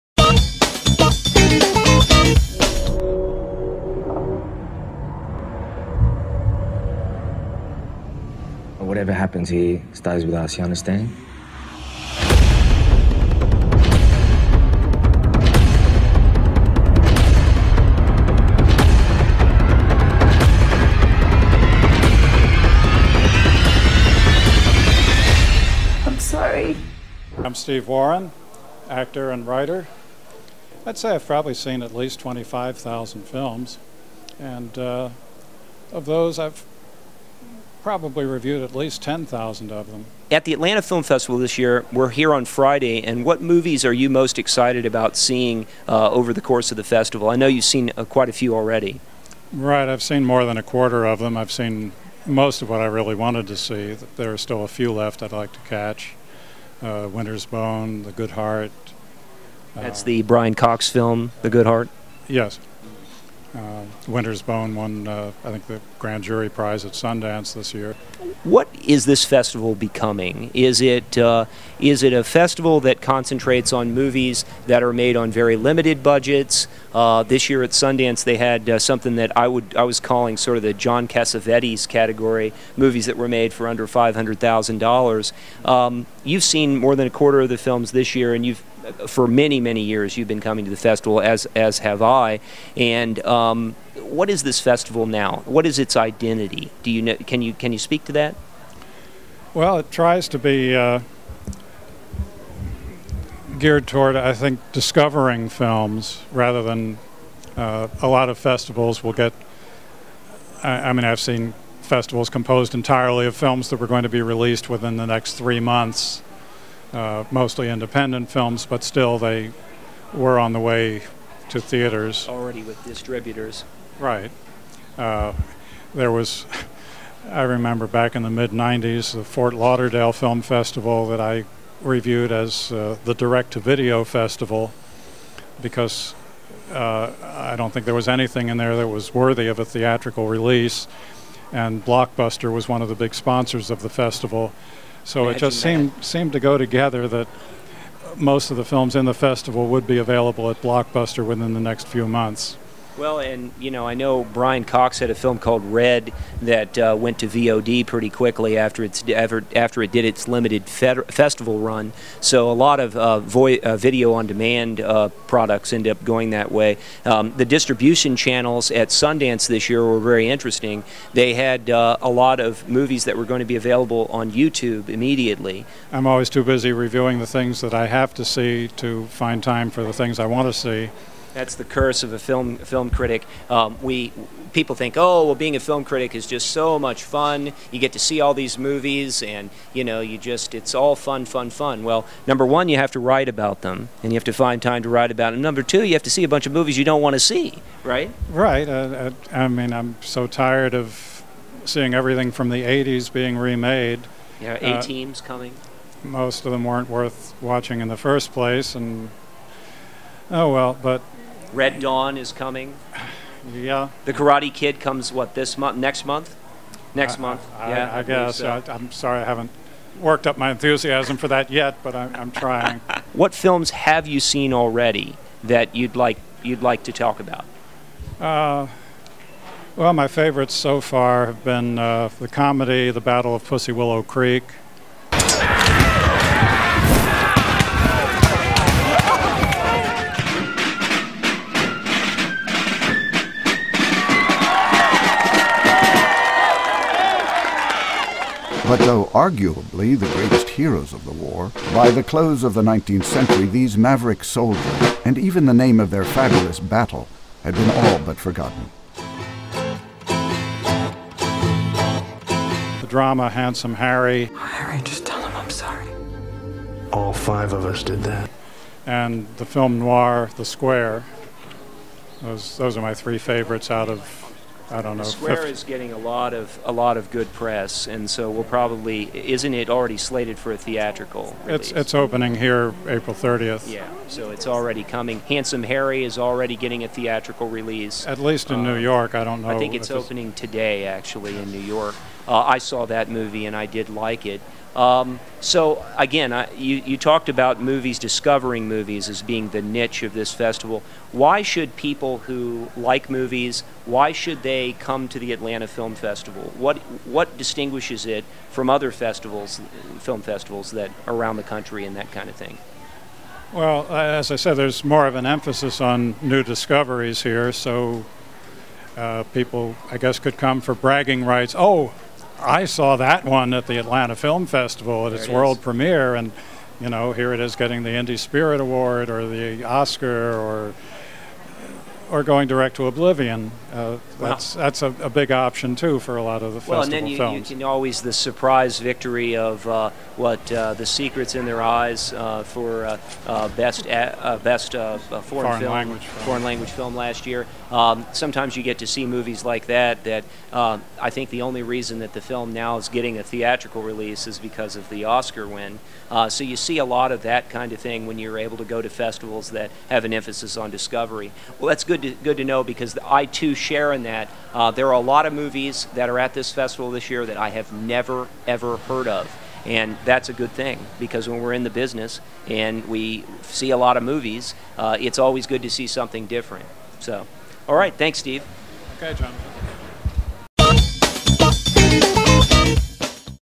The Film Fix: Atlanta Film Festival 2010 Part 1
In this first segment from the 2010 Atlanta Film Festival Jonathan sits down with veteran Atlanta film critic, writer, and actor Steve Warren.  Jonathan and Steve discuss the identity of the Atlanta Film Festival and its place on the circuit.  Filmmaker interviews will be posted all week and the Film Fix covers the AFF!